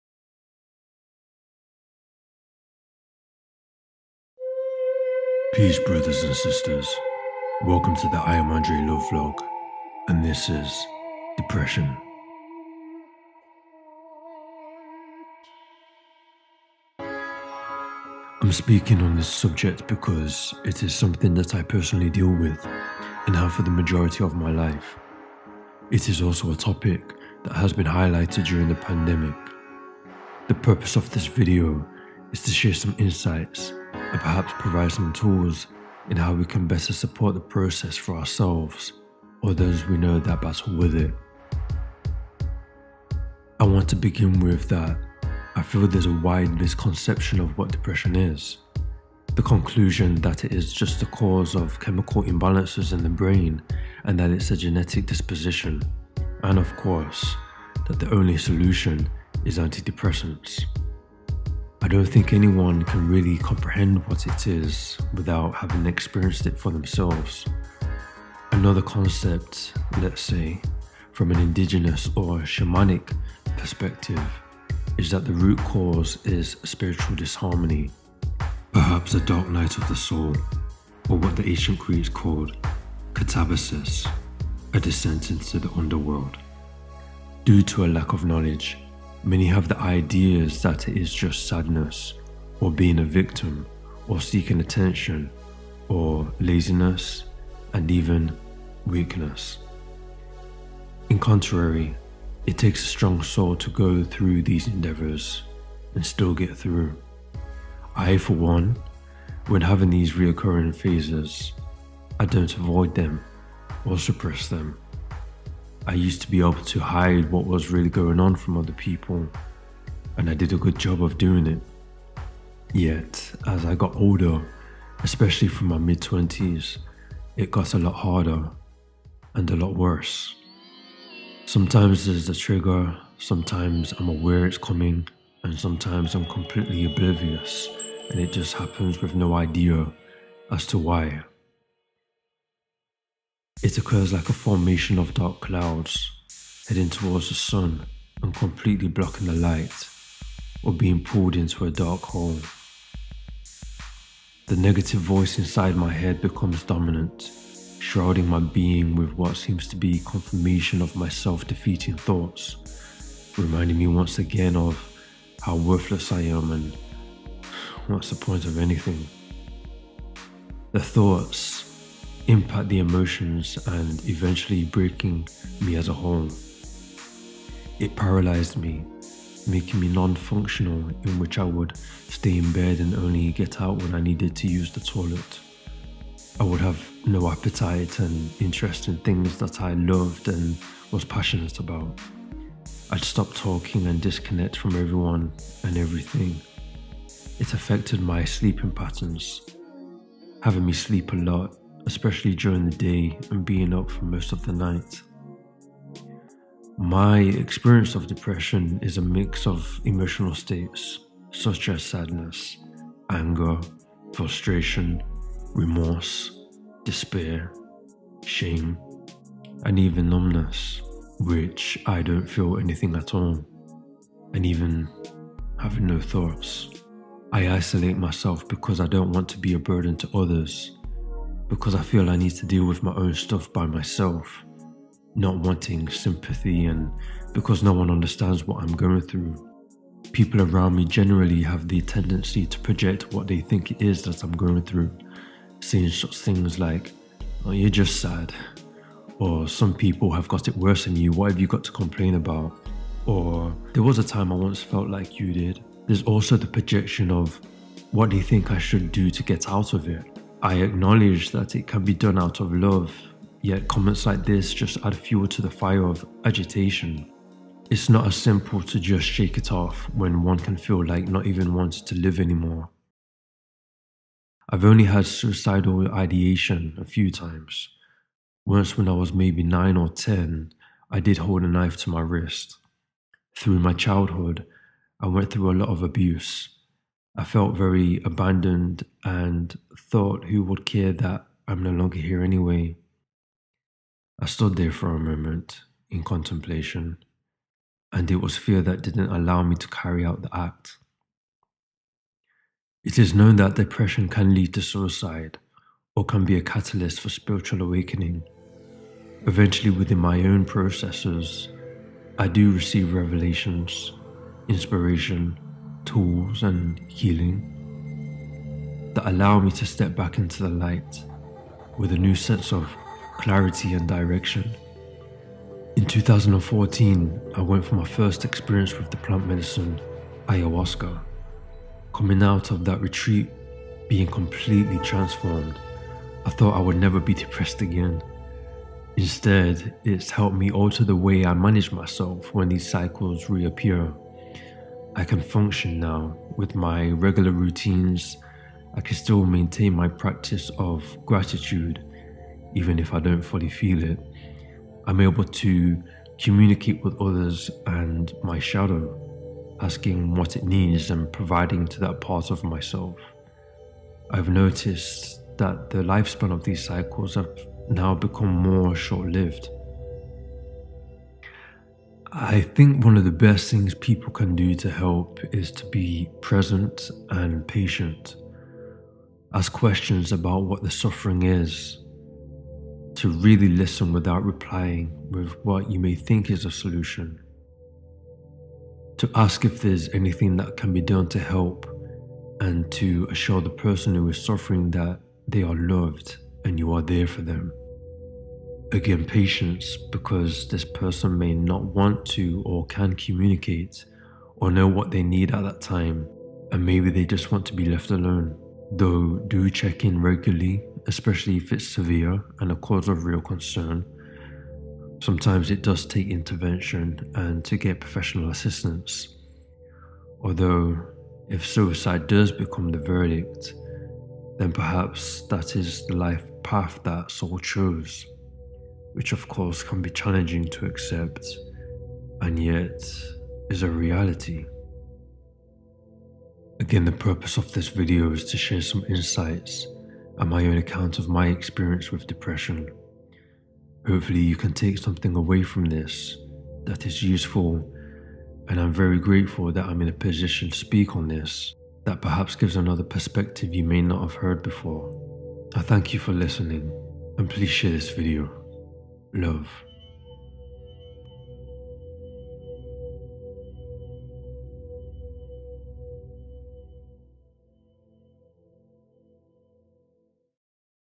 DEPRESSION - Embracing The Shadow
Sharing some insights of my own account with depression, that have common, related experiential traits. The purpose of this video, is for the potentiality of our relationship with mental health illnesses; can be revolutionised in how we treat them within ourselves or those we know that suffer.

If this resonates, please share with your loved ones.

Love ☺️🙏🏾🌿

All music by me